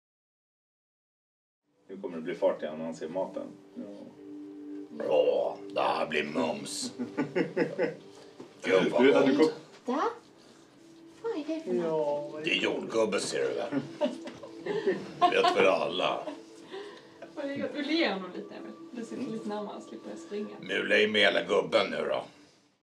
Under Täcket - Kennedy Pratar
HeatoNs hund Kennedy kan tydligen prata. Vad säger han? :D